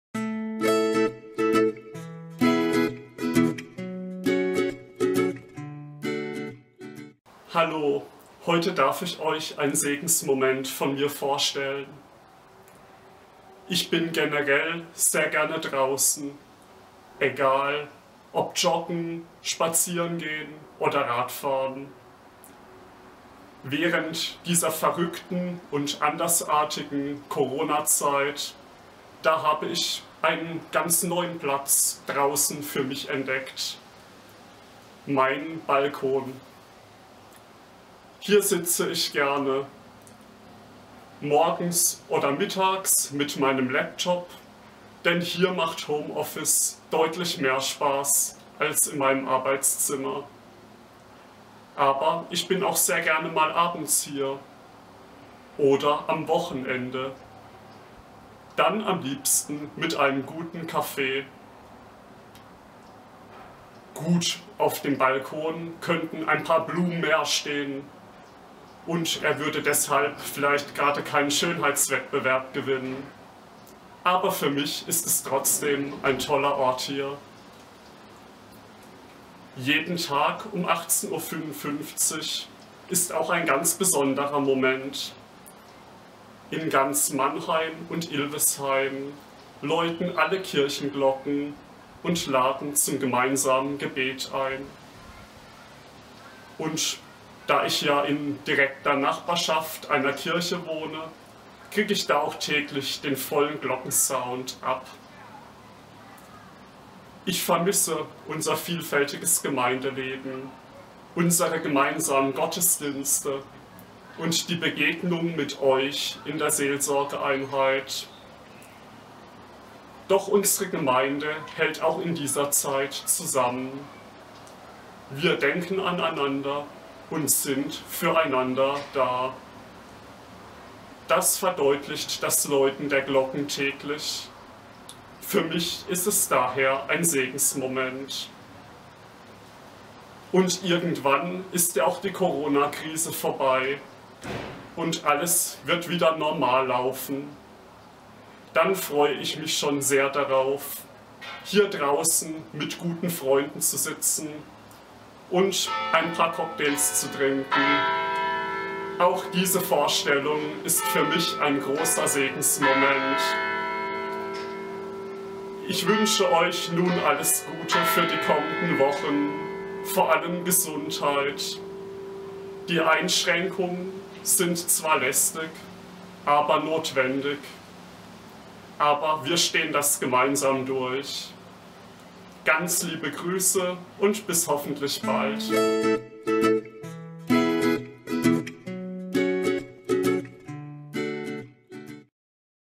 Segensmoment 05
Hallo zusammen, heute erzählt uns Sebastian Binder von seinem Segensmoment.